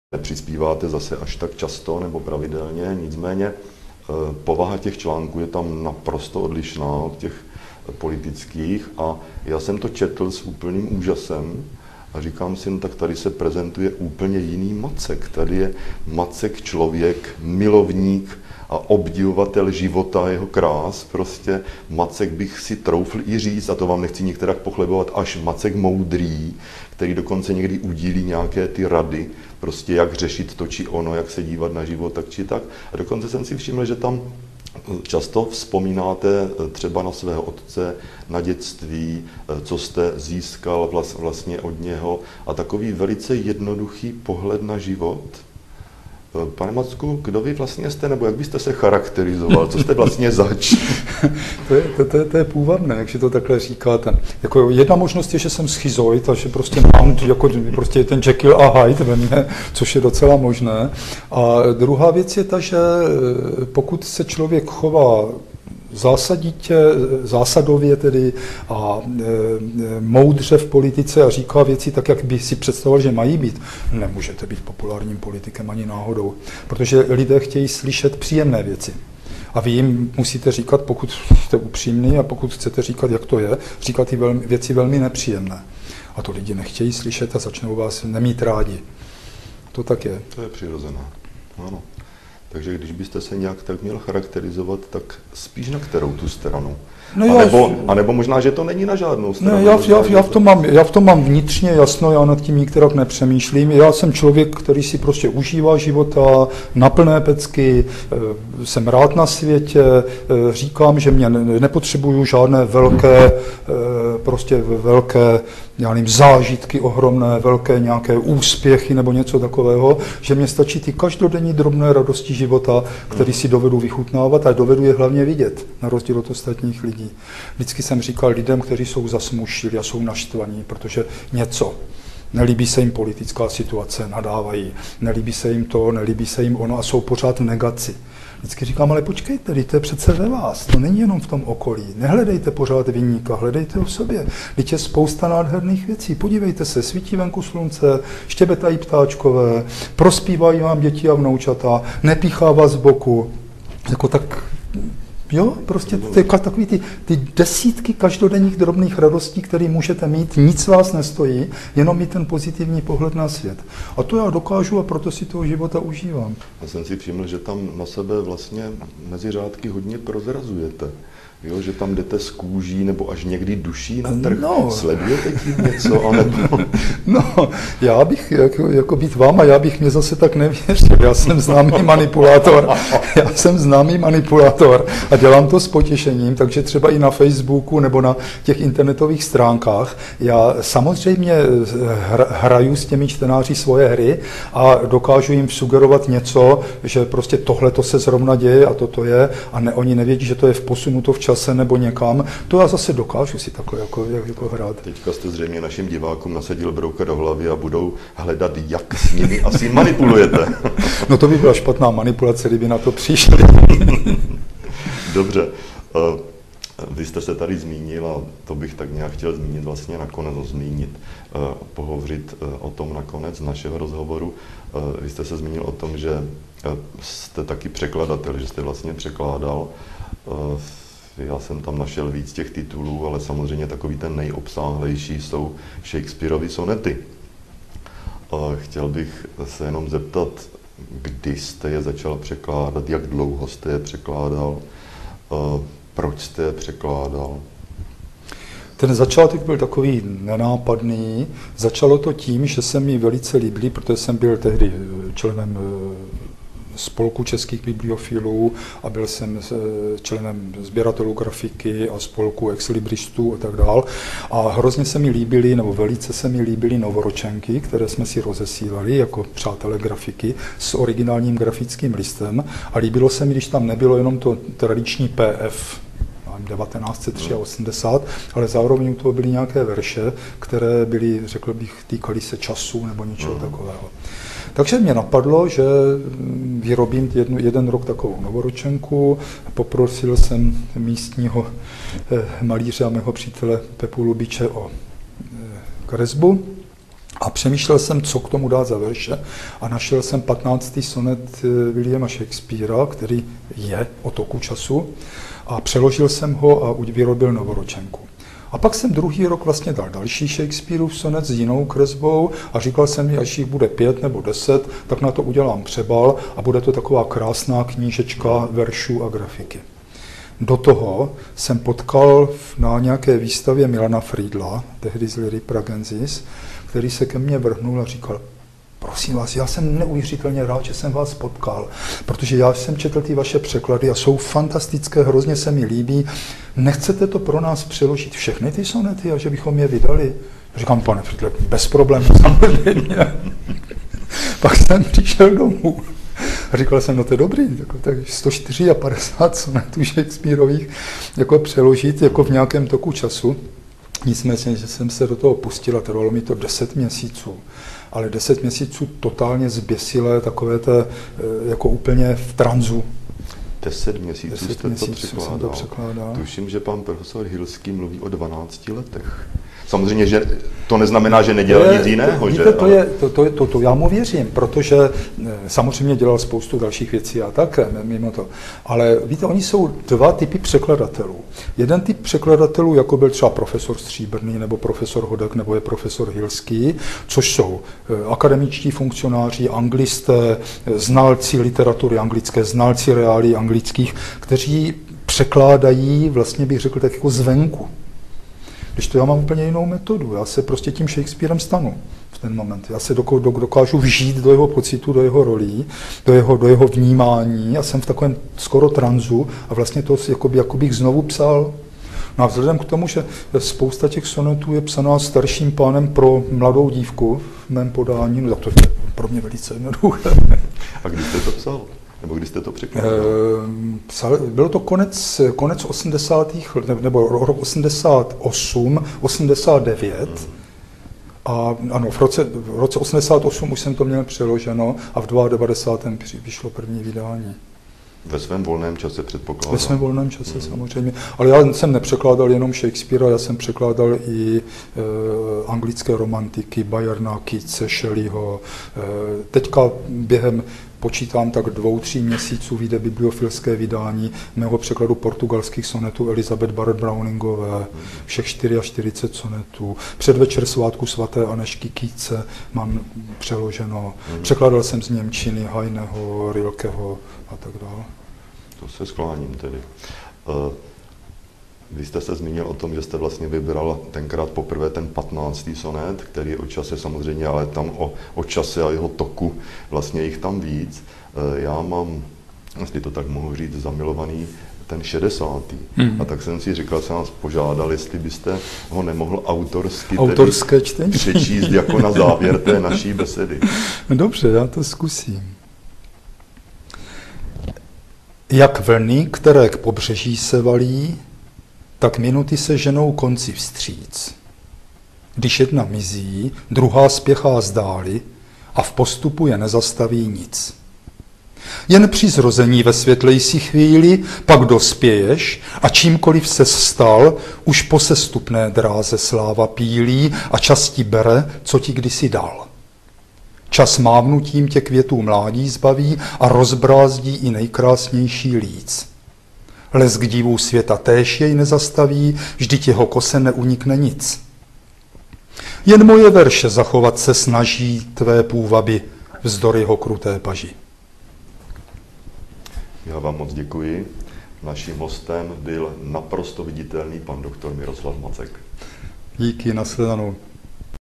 Beseda s Miroslavem Mackem část 2
Do našeho televizního studia přijal pozvání rodák z regionu Zábřežska - Miroslav Macek.